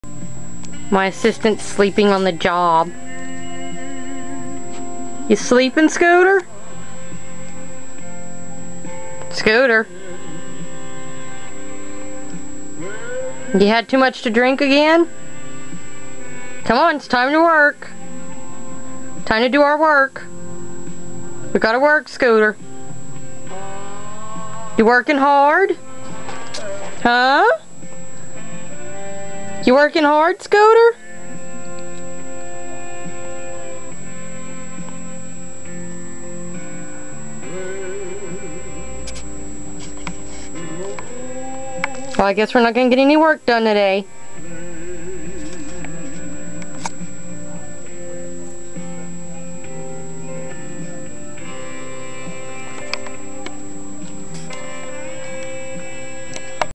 Scooter working hard or hardly working.
My 3 legged cat Scooter is my sculpting/doll making assistant,but sometimes he drinks to much and passes out in the middle of his work.